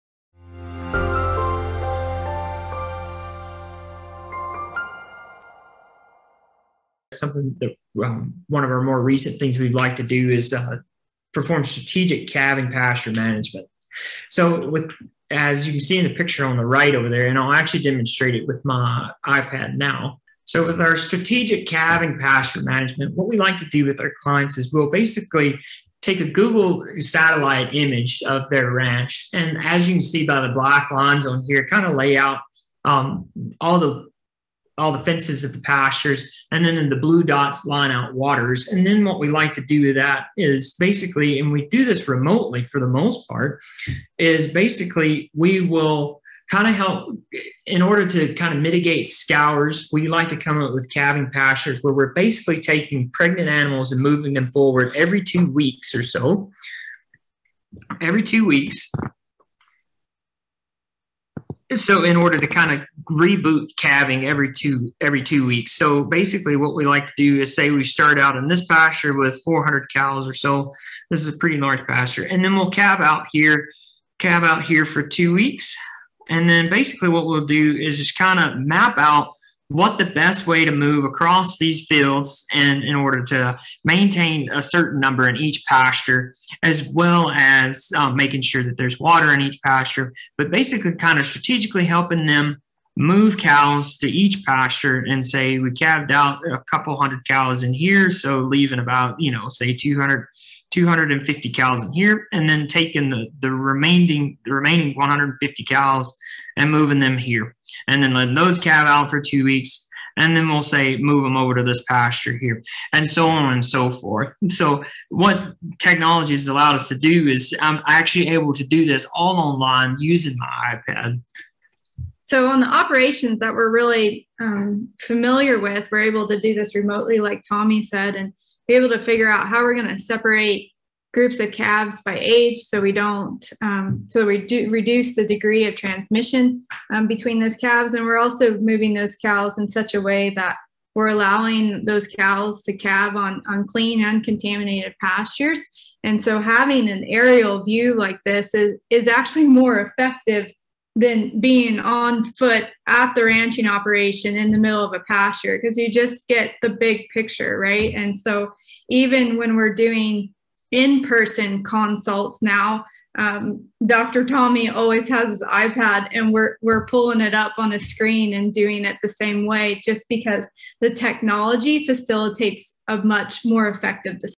How to simplify planning for strategic calving pasture management using available technology
In this clip from BovInnovation held during the virtual Canadian Beef Industry Conference in 2021, Dr. Tommy Ware and Dr. Elizabeth Homerosky make planning for strategic calving management (Sandhills Calving Management) simpler with an iPad and Google Satellite imaging. Planning your calving pasture moves can reduce scours outbreaks and improve the overall health of the herd.

🎬 VIDEO GUIDE:
0:00 Title and introduction
0:19 Using Google Satellite images to plan calving pasture movement
0:52 Frequency of moving pregnant cows (every 2-weeks)
2:08 Separating cow-calf pairs effectively to reduce disease transmission

🍁 Find more tips for Sandhills Calving Management on the BCRC Blog